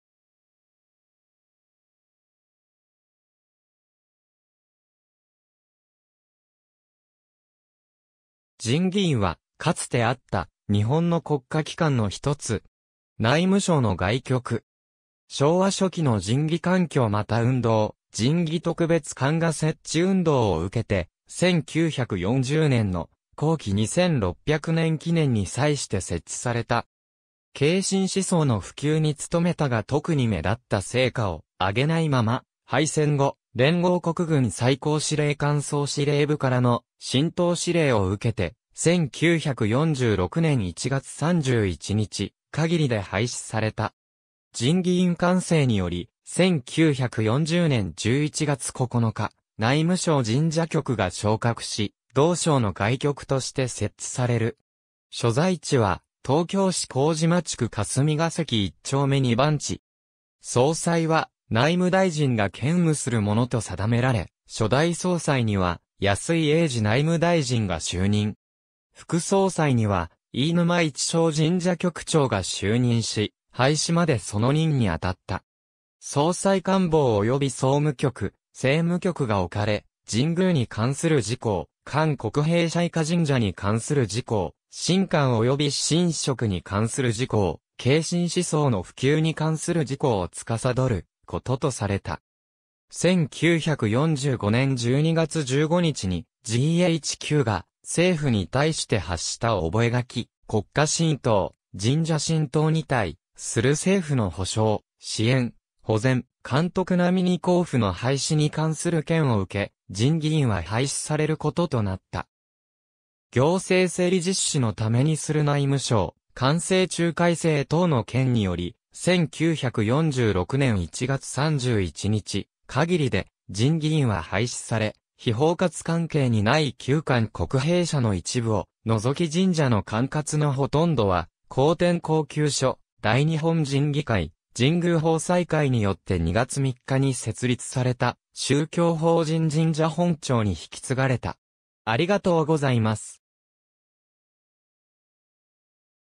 神祇院
内務省_(日本)
神祇院（じんぎいん）は、かつてあった日本の国家機関のひとつ。
内務省の外局。
昭和初期の神祇官興復運動、神祇特別官衙設置運動を受けて、1940年（昭和15年）の皇紀2600年記念に際して設置された。
敬神思想の普及に努めたが特に目立った成果をあげないまま、敗戦後、連合国軍最高司令官総司令部（GHQ）からの神道指令を受けて、1946年（昭和21年）1月31日限りで廃止された。
神祇院官制（昭和15年勅令第736号）により、1940年(昭和15年)11月9日、内務省神社局が昇格し、同省の外局として設置される。
所在地は東京市麹町区霞ヶ関一丁目二番地（内務省庁舎）。
総裁は内務大臣が兼務するものと定められ、初代総裁には安井英二内務大臣が就任。
副総裁には、飯沼一省神社局長が就任し、廃止までその任に当たった。
総裁官房及び総務局、政務局が置かれ、神宮に関する事項、官国幣社以下神社に関する事項、神官および神職に関する事項、敬神思想の普及に関する事項を掌ることとされた。
1945年（昭和20年）12月15日にGHQが政府に対して発した覚書「国家神道、神社神道ニ対スル政府ノ保証、支援、保全、監督並ニ弘布ノ廃止ニ関スル件」(SCAPIN-448)を受け、神祇院は廃止されることとなった。
行政整理実施ノ為ニスル内務省官制中改正等ノ件（昭和21年勅令第59号）により、1946年(昭和21年)1月31日限りで神祇院は廃止され、 被包括関係にない旧官国幣社の一部を除き神社の管轄の殆どは皇典講究所、大日本神祇会、神宮奉斎会によって2月3日に設立された宗教法人神社本庁に引き継がれた。